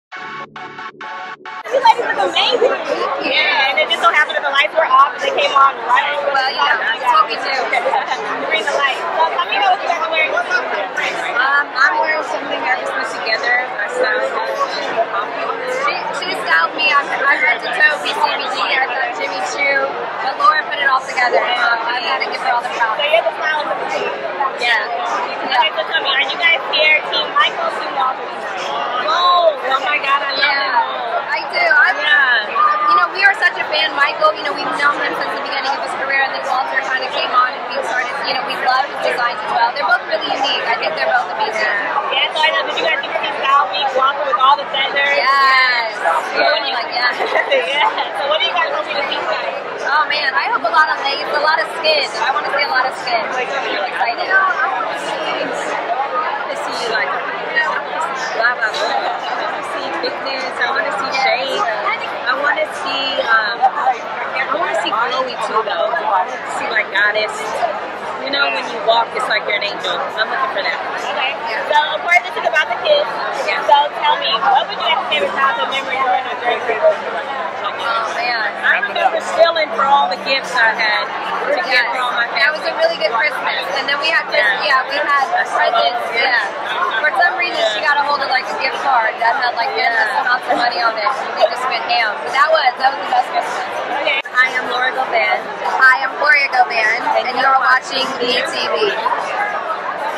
Runway Wonderland | Gloria and Laura Govan
Gloria and Laura Govan talk about what their wearing along with what they both expect to see from Michael Costello during his showcase at Runway Wonderland